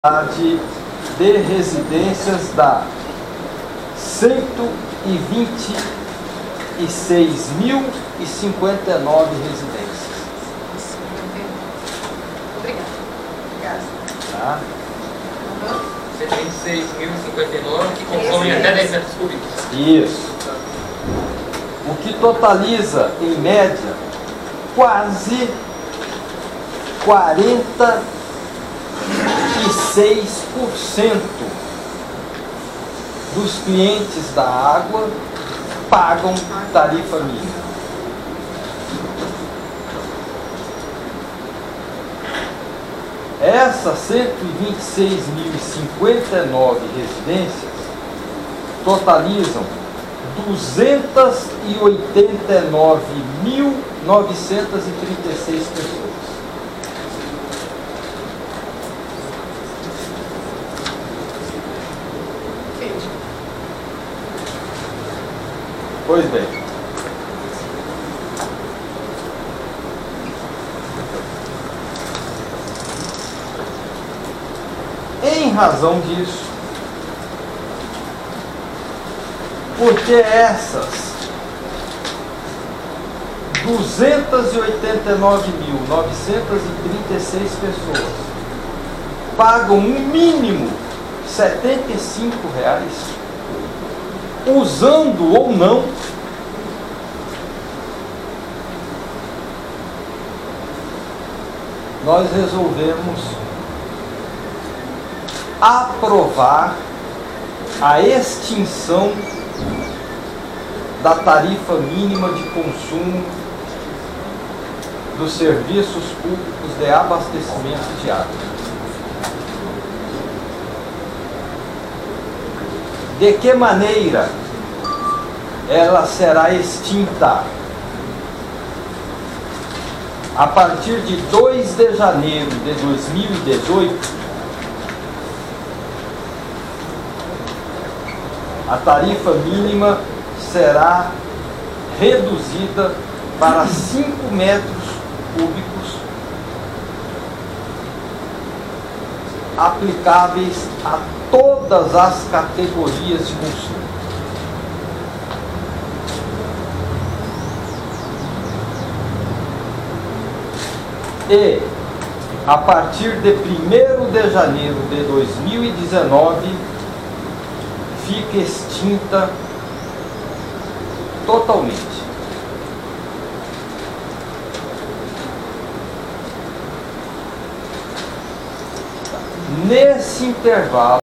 Entrevista coletiva: Prefeito acaba com a tarifa da taxa mínima nas contas de água e esgoto
O prefeito Marcos Trad (PSD) e o diretor-presidente da Agência de Regulação de Serviços Públicos (Agereg), Vinícius Campos Leite, promoveram hoje pela manhã no gabinete do chefe do executivo municipal uma coletiva de imprensa para explicar a extinção da tarifa mínima cobrada nas contas de água em Campo Grande. Na prática funcionaria assim: Quem utiliza menos que 10 m³ de água por mês, deixa de pagar a tarifa mínima e passa a pagar, progressivamente, exatamente o valor consumido.
Medida inédita no País
Após oito meses de estudos técnicos, a prefeitura certificou-se que 130 mil residências da capital, ou em média 300 mil pessoas pagam R$ 75 pela água e esgoto. Com a mudança, já no primeiro ano, as famílias passarão a pagar R$ 40, tal medida resultará em 53% de economia aos contribuintes envolvidos, isso já em 2018, e em 2019 cem por cento.
O prefeito fez questão de frisar que os pobres não vão continuar a pagar as contas dos mais ricos e que a Águas Guariroba, se sentir prejudicada que procure os meios legais, estamos aberto ao dialogo sempre, no ano passado a Águas Guariroba  contabiliza um lucro mensal de R$ 5 milhões com a política da tarifa mínima.